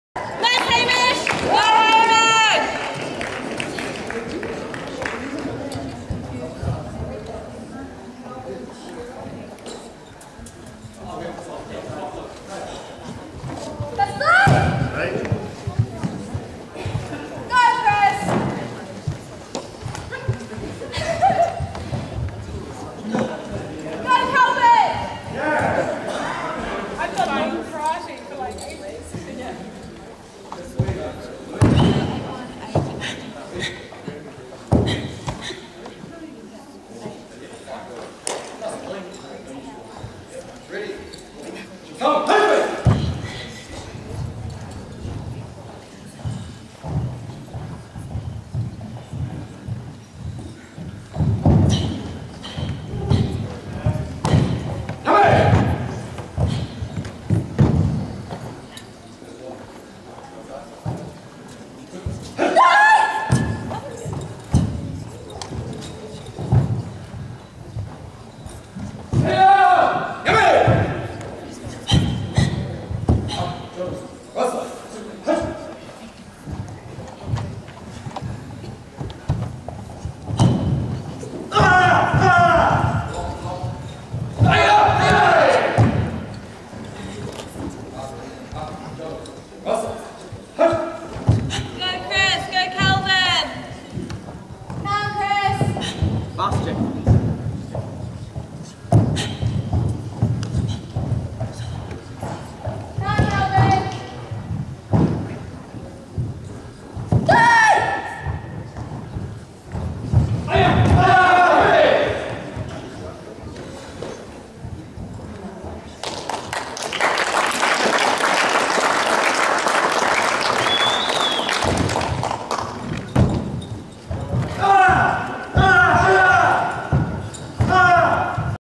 gkr chch r2 05 09 2015 rg1 jnr open female kata 06d bassai dai
Phillipstown

05 September 2015

Ring1 Junior Open Female Kata & Kumite